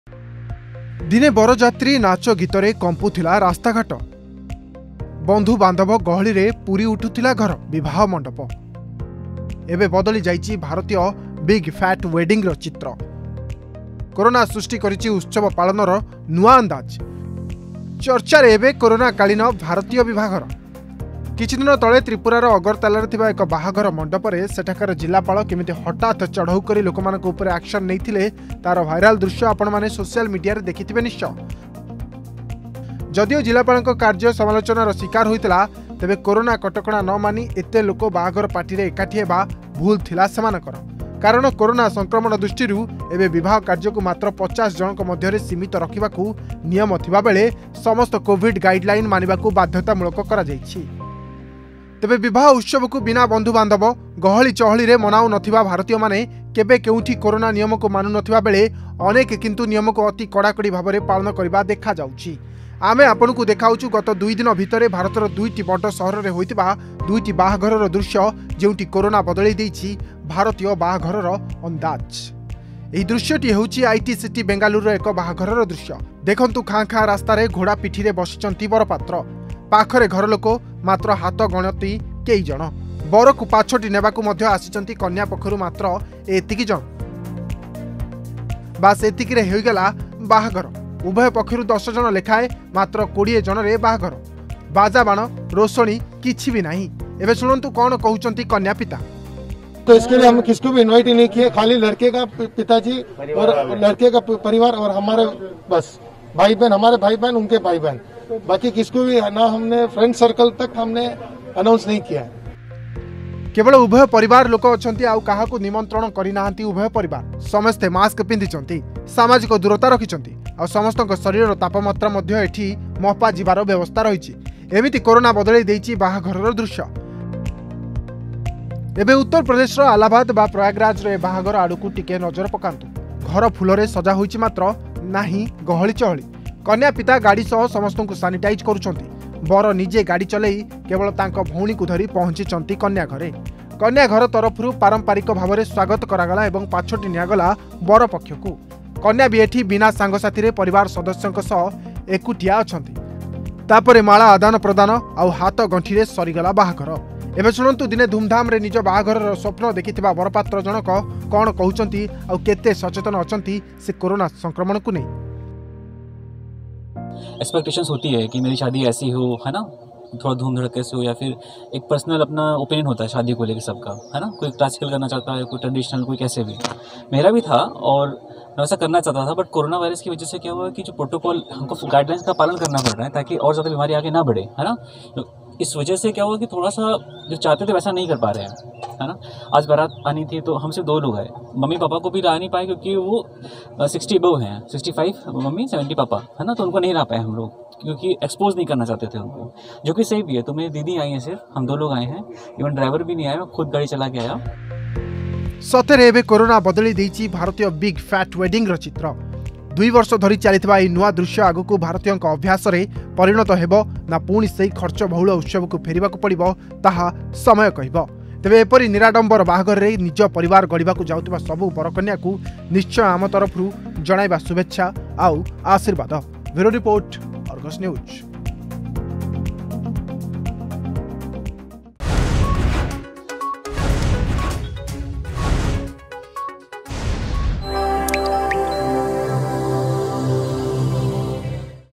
ଦିନେ ବରଯାତ୍ରୀ, ନାଚଗୀତରେ କମ୍ପୁଥିଲା ରାସ୍ତାଘାଟ
ଦିନେ ବରଯାତ୍ରୀ, ନାଚଗୀତରେ କମ୍ପୁଥିଲା ରାସ୍ତାଘାଟ. ବନ୍ଧୁବାନ୍ଧବ ଗହଳିରେ ପୁରି ଉଠୁଥିଲା ଘର, ବିବାହ ମଣ୍ଡପ. ଏବେ ବଦଳି ଯାଇଛି ଭାରତୀୟ ବିଗ୍‌ଫ୍ୟାଟ୍ ୱେଡ଼ିଙ୍ଗ୍‌ର ଚିତ୍ର. କରୋନା ସୃଷ୍ଟି କରିଛି ଉତ୍ସବ ପାଳନର ନୂଆ ଅନ୍ଦାଜ...